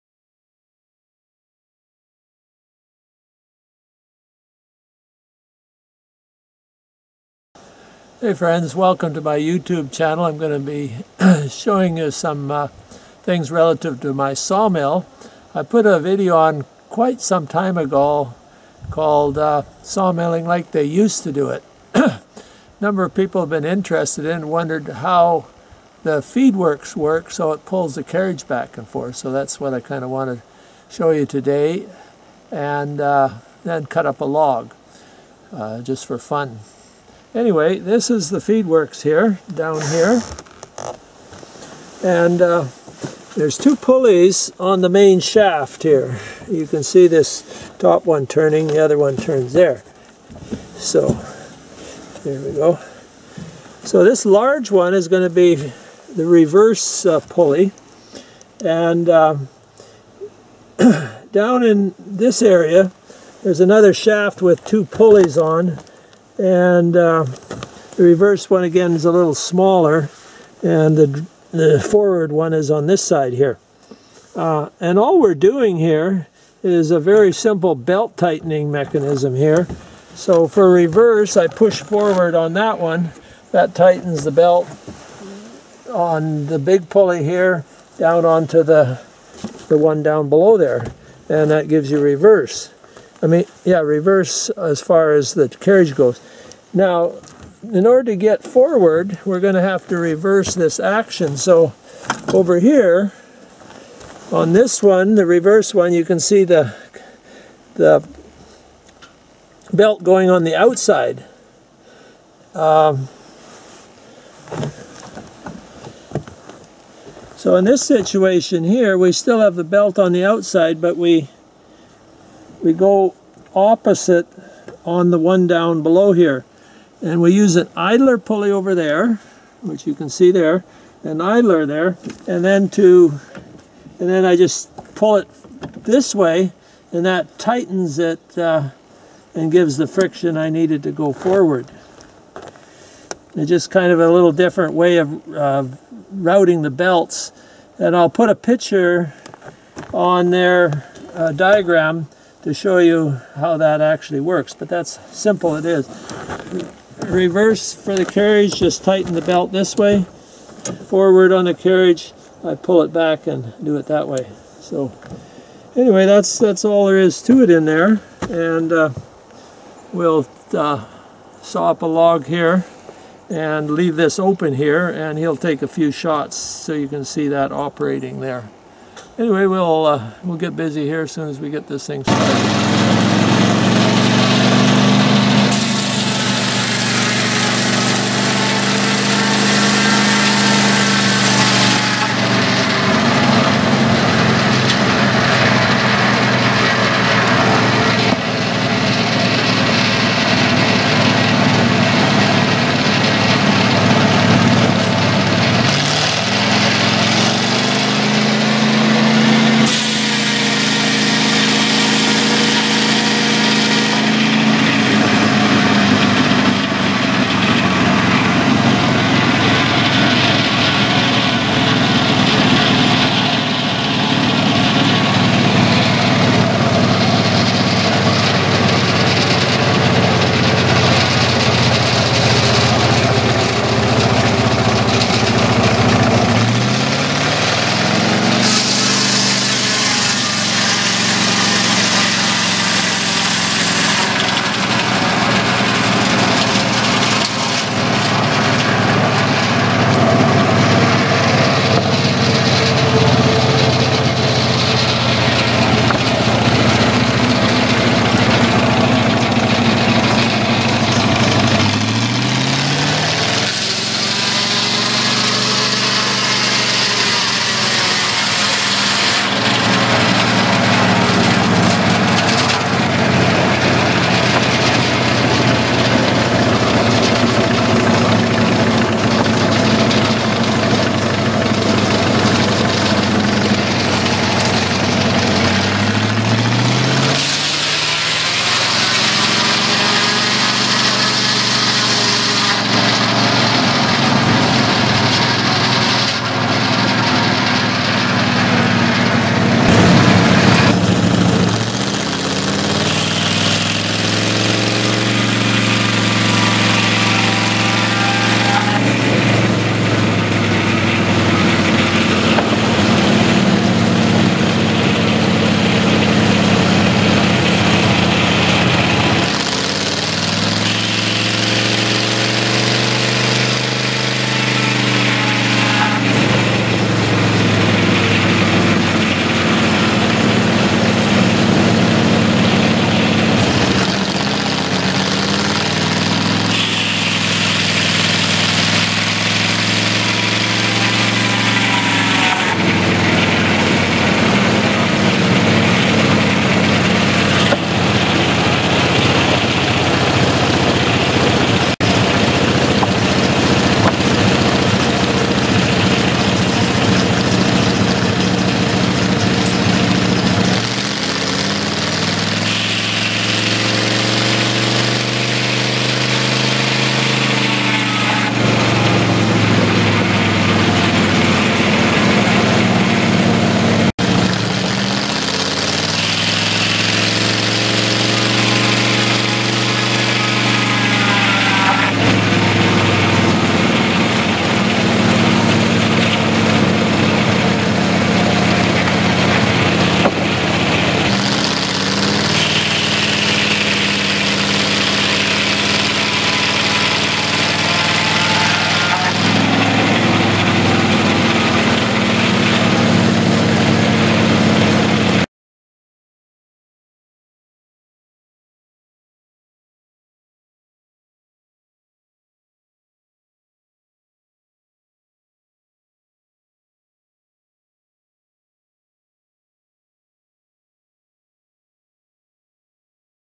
SAWMILLING LIKE THEY USE TO DO IT, CARRIAGE FEED WORKS
In this video I show you how the feedworks pulls the carriage back and forth on my sawmill. If you haven't watched the first one you may want to do that. Sawmilling like they use to do it.  An old time Saw mill that I made from scrap parts.